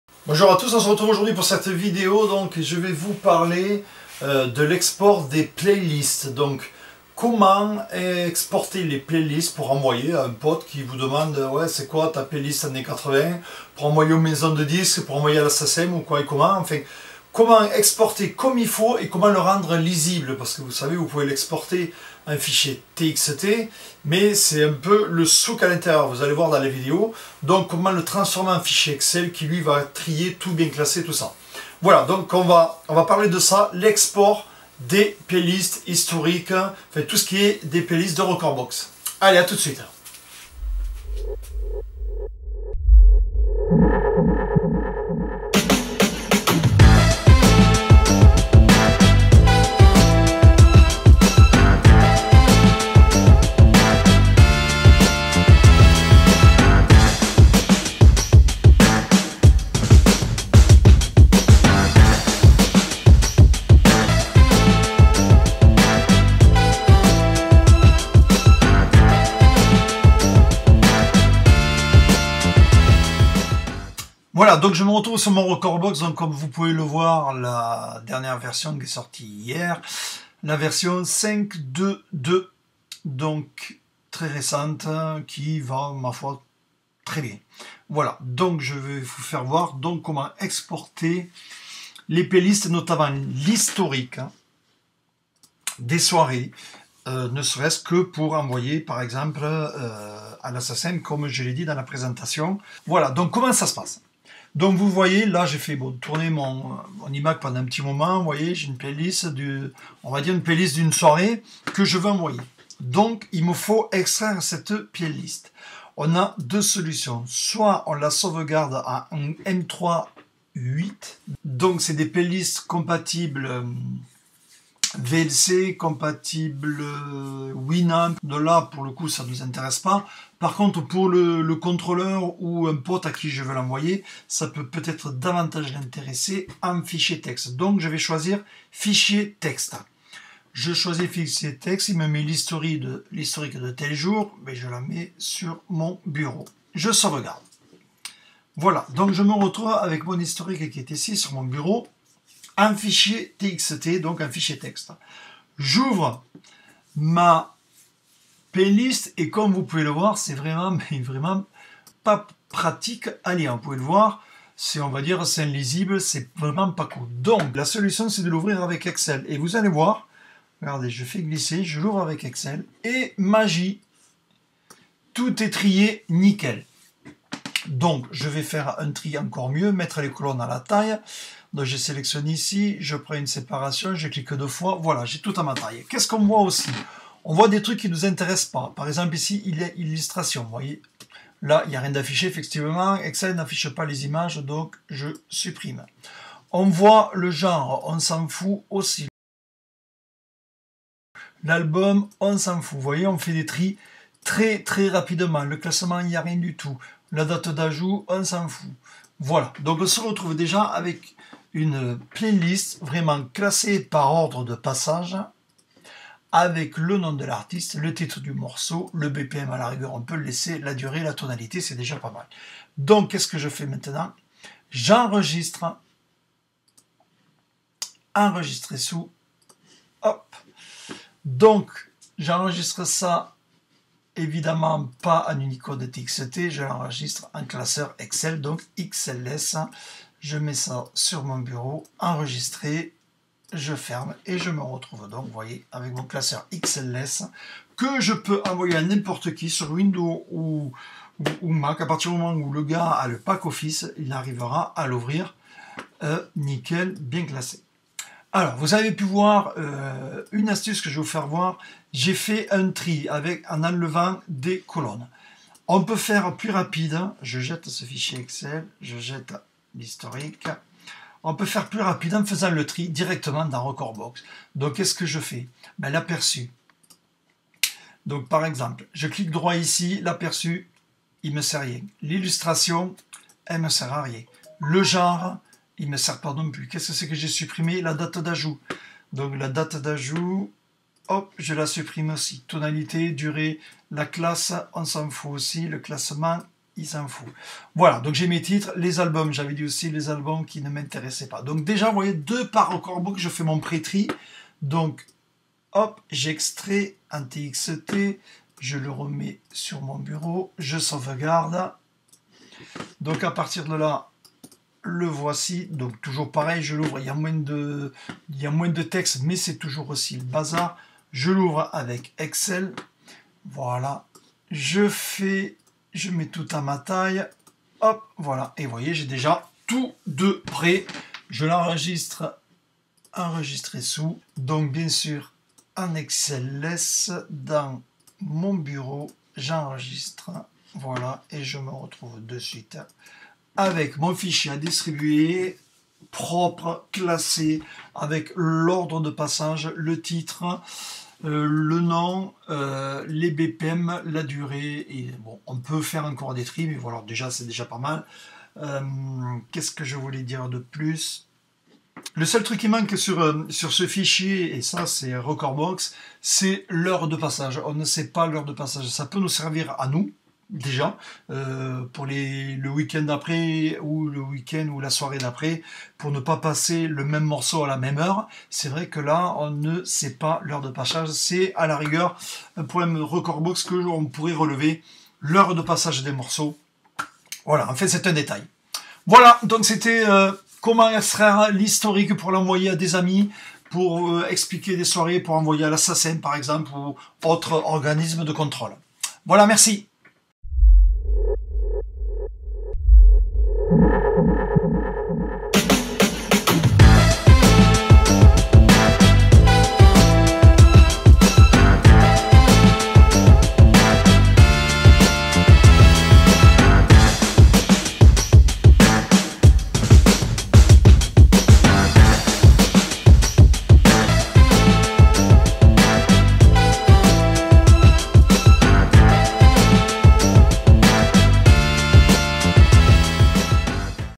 Video 71: Export playlist "historique" "etc" en TXT ou EXCEL
Comment exporter une playlist historique etc en fichier txt ou excel